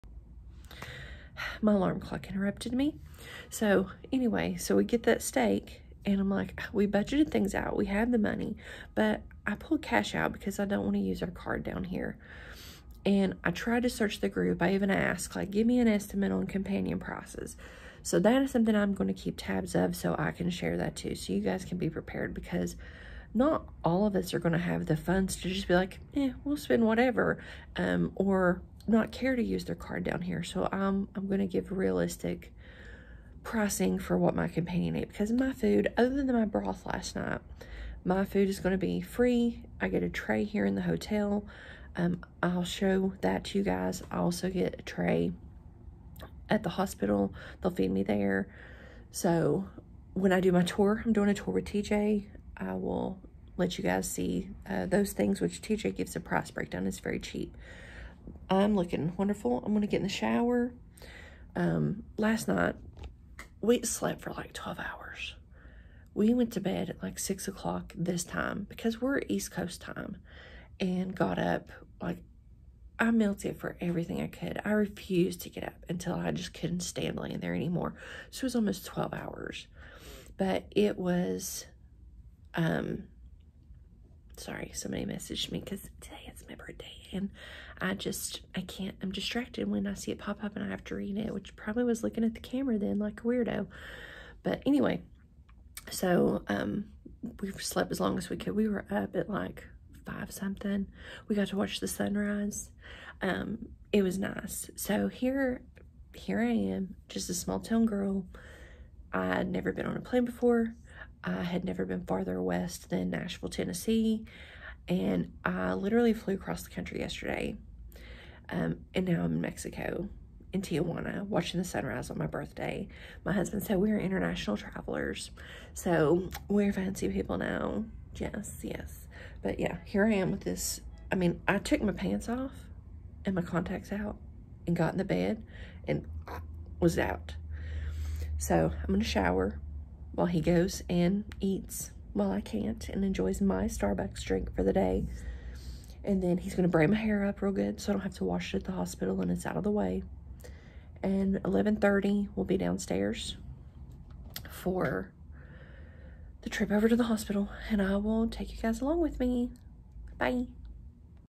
Arrival to Tijuana Part 2 | July 28, 2023
Vlog for the morning of surgery. My thoughts after arriving to Tijuana. Part 2 after my alarm interrupted the first video.

Any rude or inappropriate comments will be removed, and the user blocked. Just be kind.